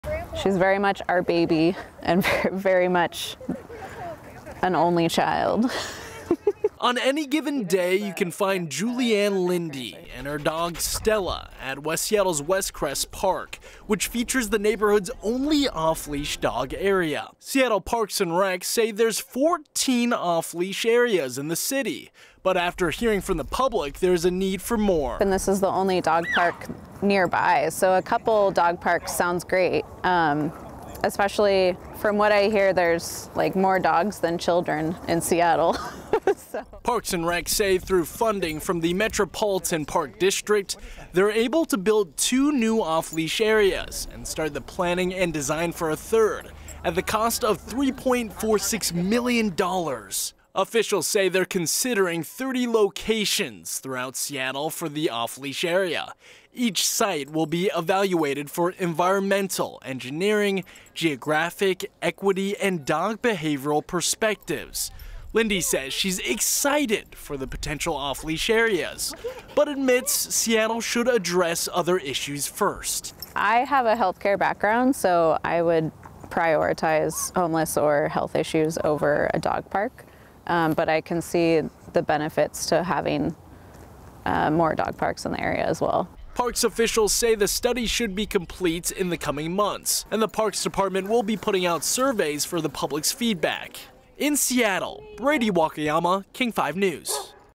Two new off-leash dog parks coming to Seattle
Seattle's four-legged friends could have two new off-leash parks to play at.

Right now, a study is being launched by the Seattle Parks and Recreation Department to find the perfect location.

On any given day you can find Julianne Linde and her dog Stella at West Seattle's Westcrest Park which features the neighborhood's only off-leash dog area. Seattle Parks and Rec said there are 14 off-leash areas in the city, but after hearing from the public there is a need for more.